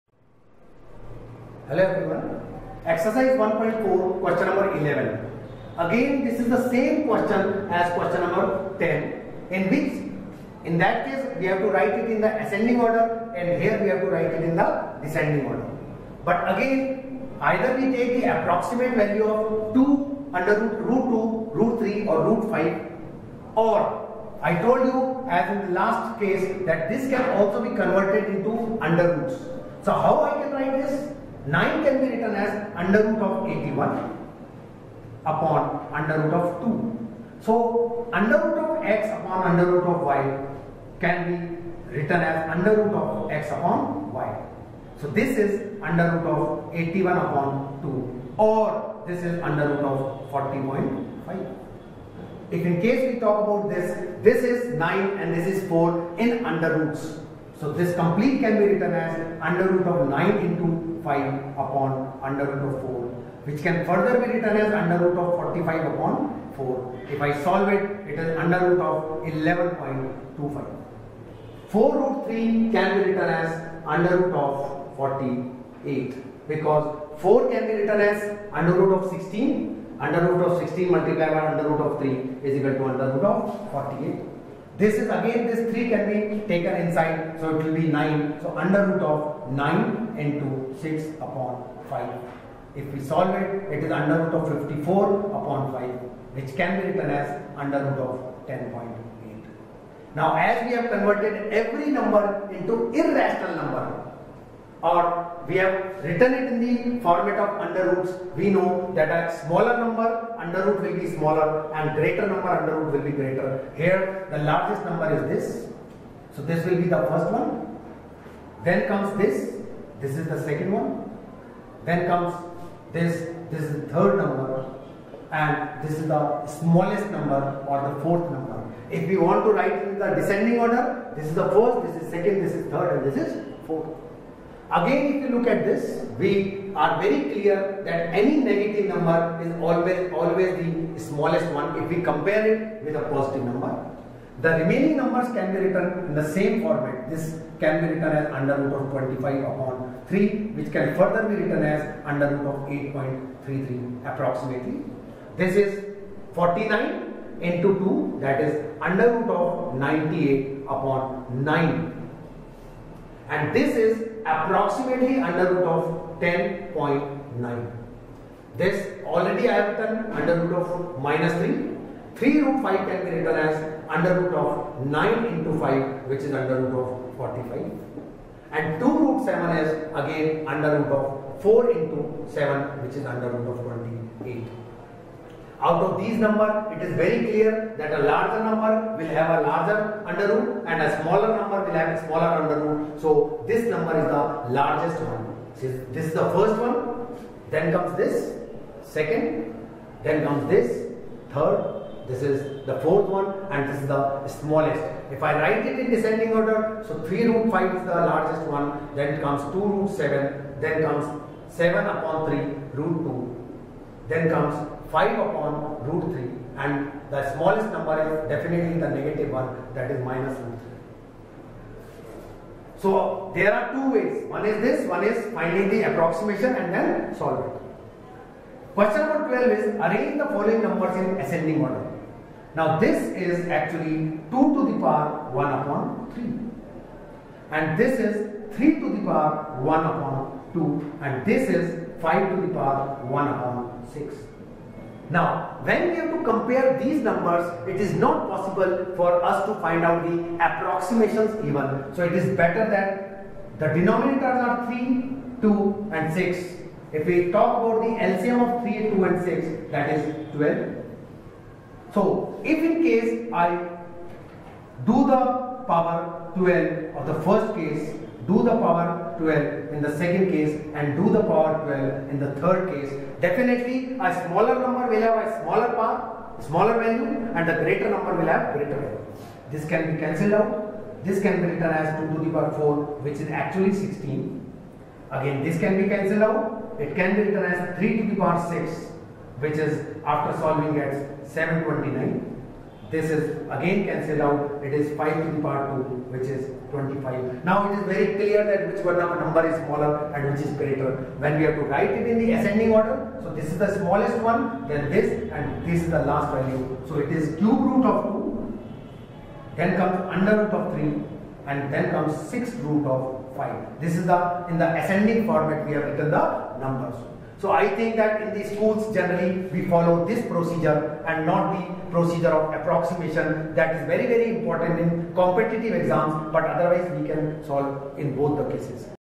Exercise 1.4 (Q11-Q12) Solution for Class 9th Understanding ICSE Mathematics by ML Aggarwal
In this video we are solving class 9th Understanding ICSE Mathematics Book by ML Aggarwal - Exercise 1.4 Question number 11 and 12.